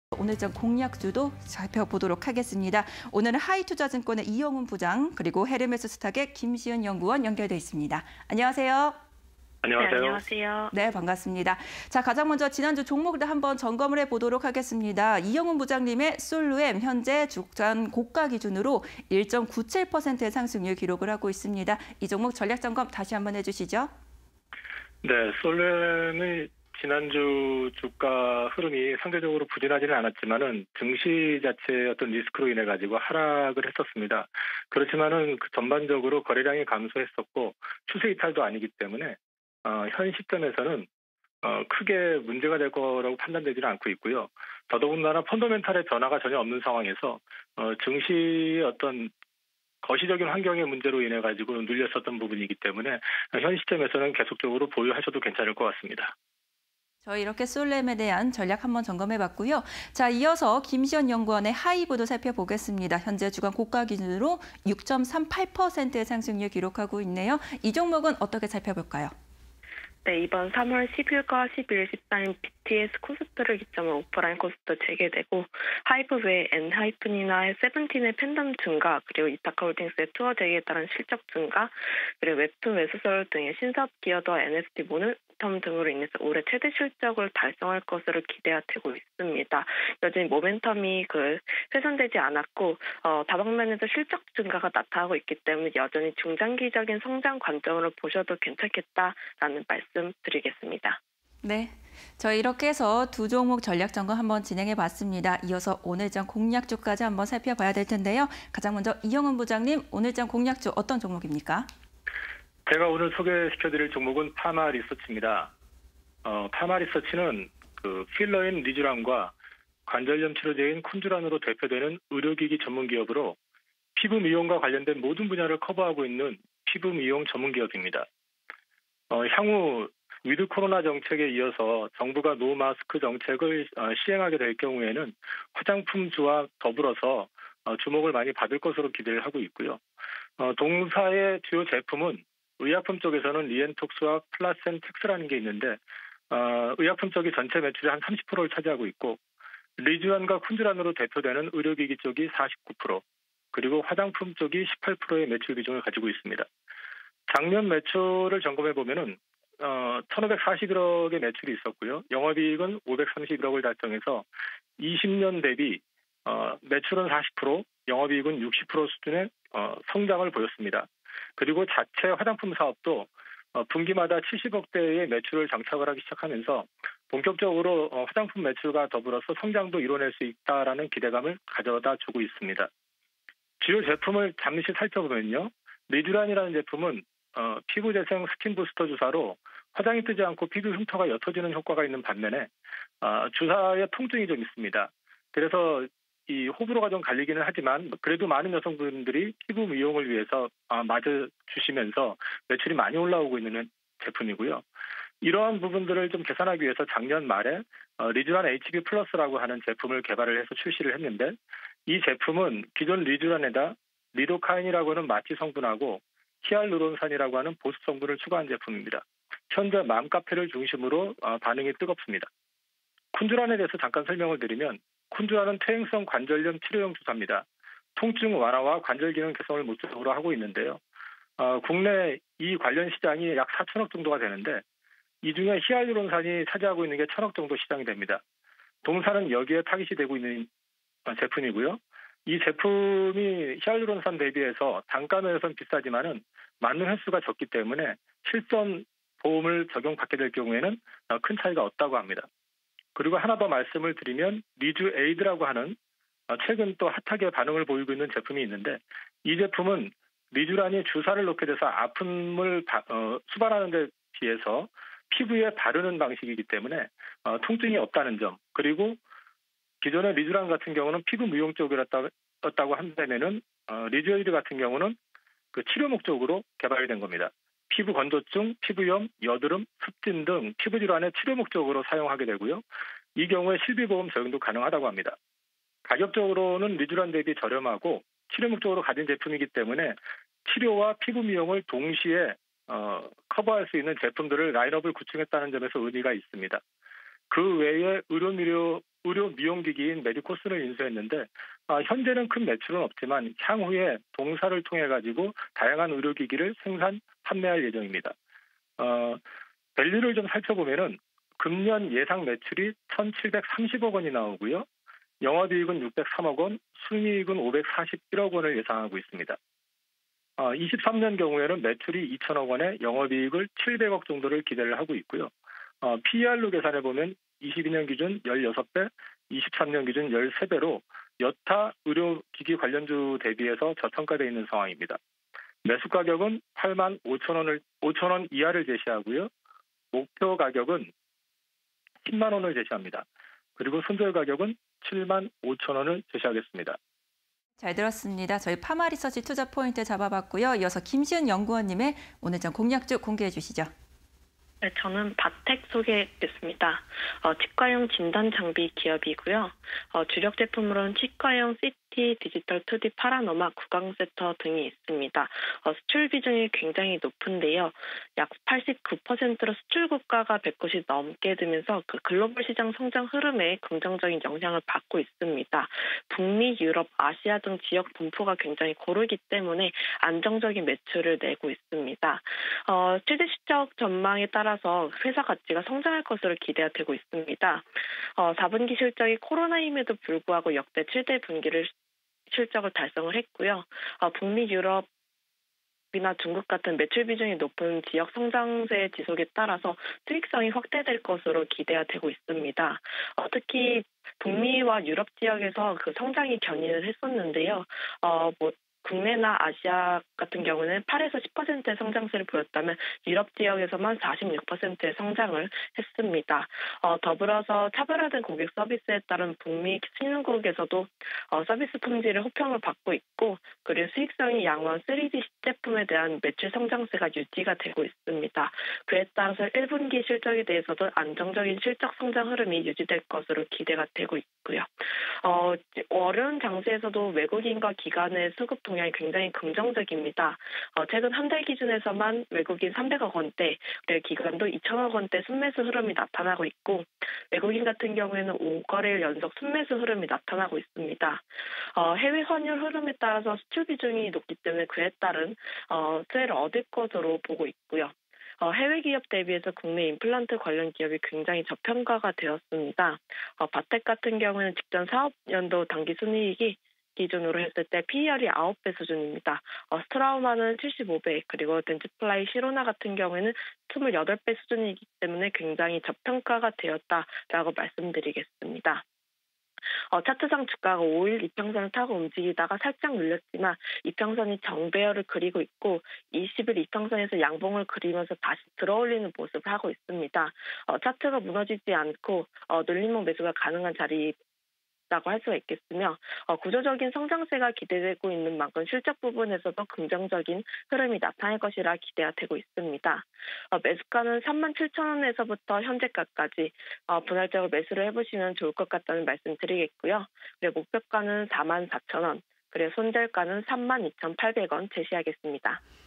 파마리서치(214450) VS 바텍(043150)_종목 VS 종목 (20220308)
파마리서치(214450)
- '리쥬란·콘쥬란' 실적 성장 지속
- 지난해 매출 40%·영업이익 60% 증가
- '리쥬에이드·리쥬란HB+' 시장 반응 긍정적
- 올해 매출 10%·영업이익 15% 이상 성장 전망

바텍(043150)
- 치과용 진단장비 기업
- 치과용 CT, 디지털 2D파노라마 제품 등 주력
- 지난해 4분기 실적, 역대 분기 최고치 달성
- 해외 수출 비중 89.6%… 글로벌 시장 성장세 긍정적
- 3D 신제품 수익성 양호… 매출 성장세 유지
- 외국인·기관 수급 동향 긍정적
- 국내 임플란트 관련 기업 저평가 상태
- 직전 사업 연도 당기순이익 기준 PER 9배 수준